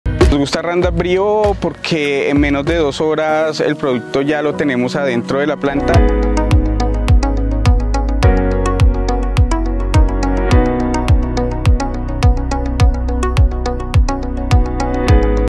Control eficiente de malezas con Roundup® Brio | Agro Bayer Colombia​
Roundup® Brio es eficiente en el control de malezas en tu cultivo de maíz.​